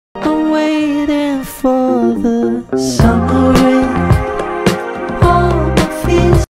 That feeling when you finally finish a challenging pattern and it turns out just right.
Isn't it amazing? The stitch is so perfect you could make probably any project with it. Share to your friends for inspiration 👌